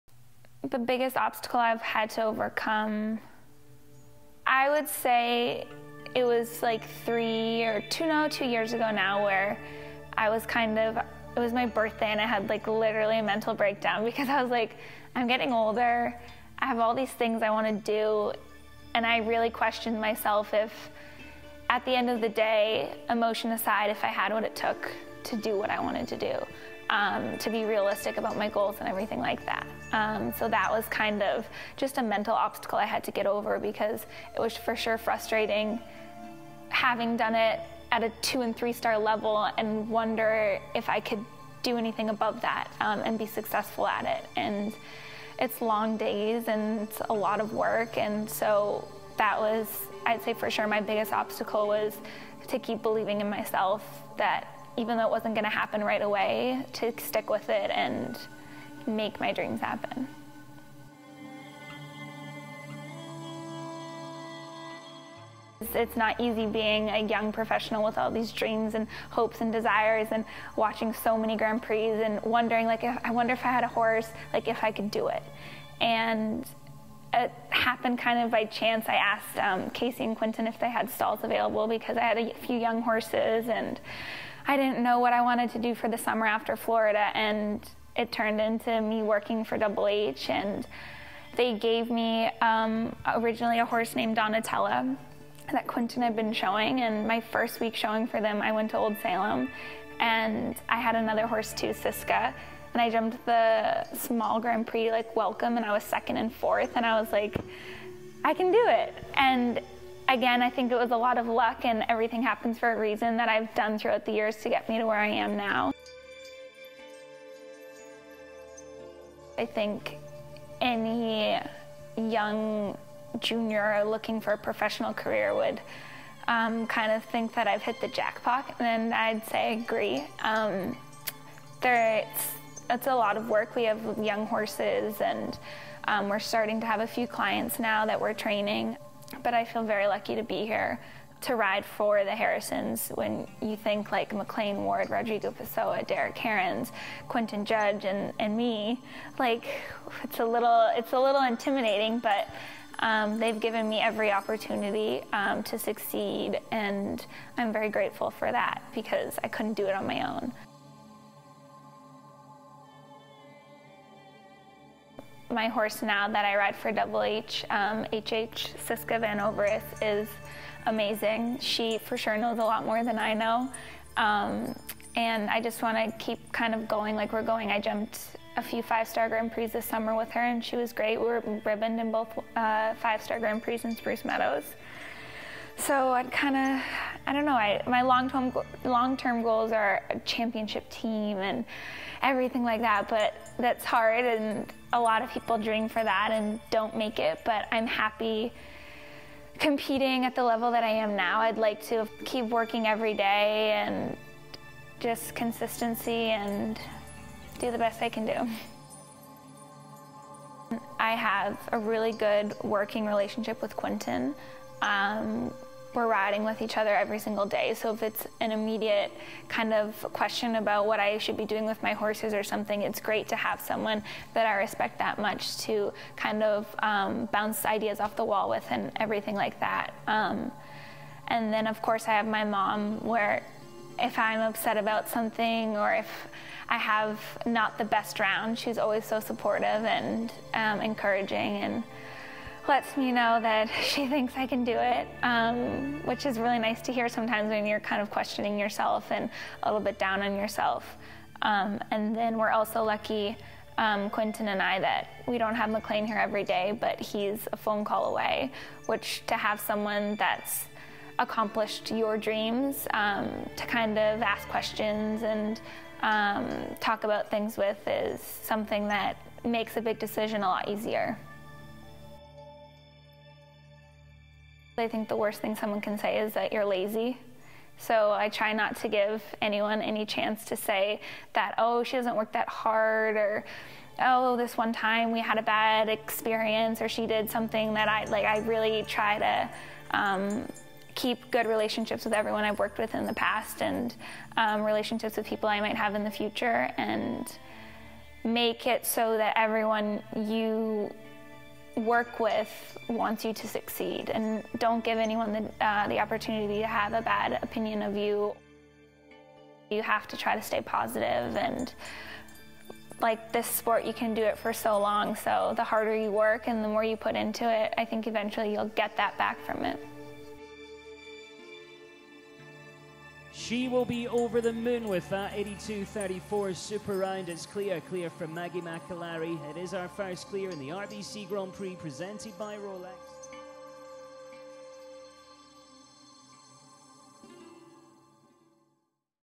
Maggie McAlary: At Present
"I think the worst thing that someone can say is that you're lazy, so I try not to give anyone any chance to say, 'Oh she doesn't work that hard...'"

Professional show-jumper Maggie McAlary talks working for HH, current horses, goals, and work ethic.

"You have to try to stay positive, and this sport, you can do it for so long, so the harder you work and the more you put into it I think eventually you'll get that back from it.”